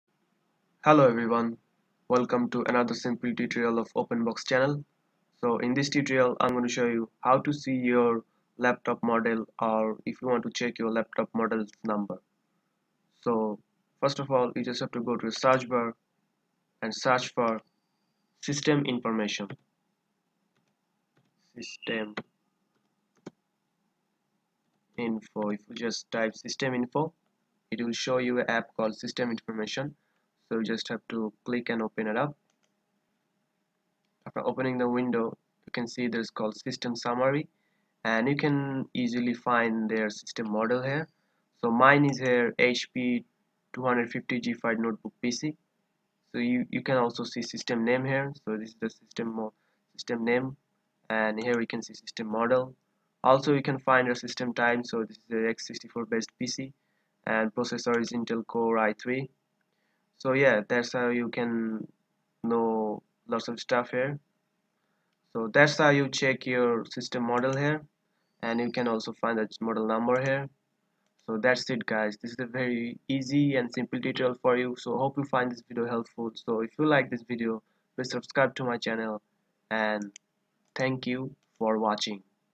How to check laptop model | Laptop model number check
in this video im going to show you how to check laptop model also
how to check laptop model windows 10 and how to check laptop model number .